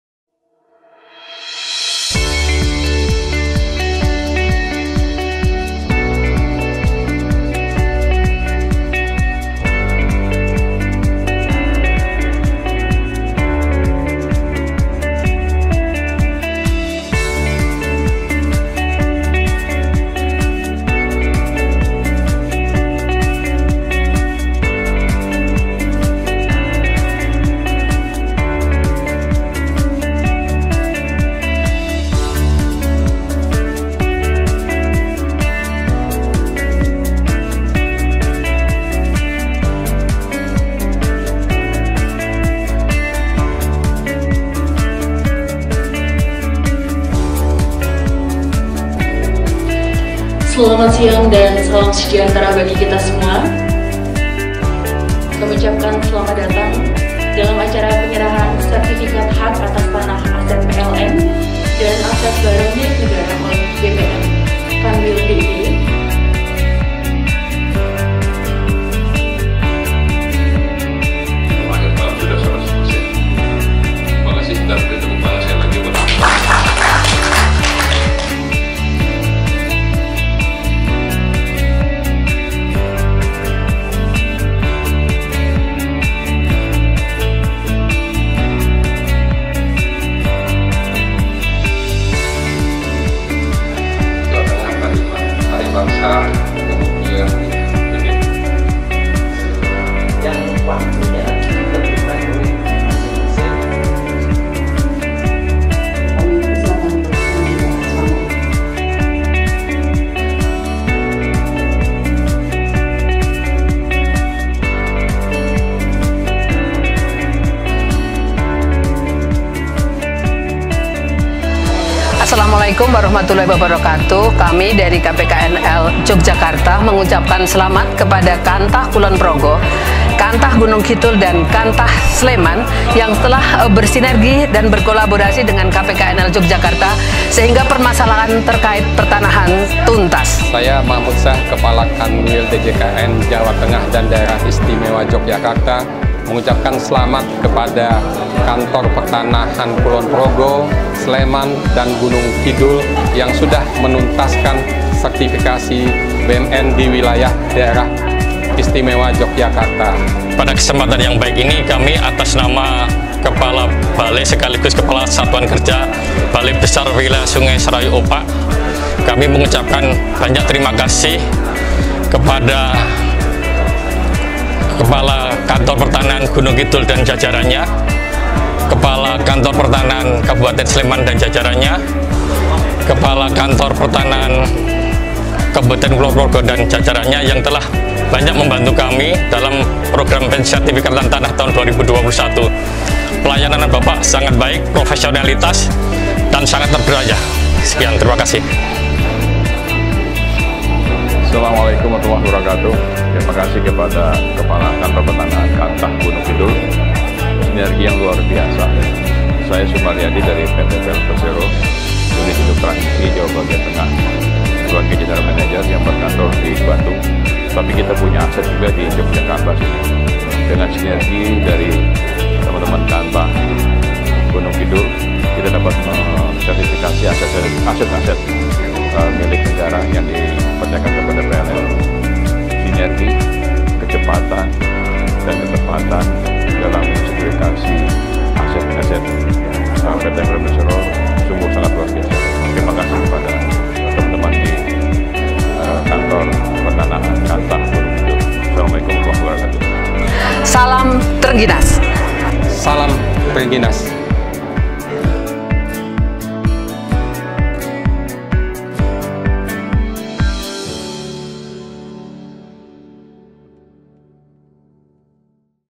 PENYERAHAN SERTIPIKAT HAK ATAS TANAH ASET TANAH BARANG MILIK NEGARA
Hallo SobATRBPN DIY,

Telah dilaksanakan Penyerahan Sertipikat Hak Atas Tanah Aset Tanah Barang Milik Negara di Eastparc Hotel Yogyakarta pada Senin, 20 September 2021.

Pada kesempatan ini dihadiri oleh Kepala Kantor Wilayah Dirjen Kekayaan Negara Jawa Tengah dan DIY, Kepala Kantor Pelayanan Kekayaan Negara dan Lelang Yogyakarta, Kepala Balai Besar Wilayah Sungai Serayu Opak di Yogyakarta, Kepala Balai Besar Pelaksanaan Jalan Nasional di Yogyakarta, Rektor Universitas Islam Negeri Sunan Kalijaga di Yogyakarta, dan Kepala Balai Pelestarian Cagar Budaya di Yogyakarta.

Sertipikat yang diserahkan sebanyak 560 sertipikat BMN yang sebagian sudah diserahkan secara langsung oleh Kantor Pertanahan masing-masing se-DIY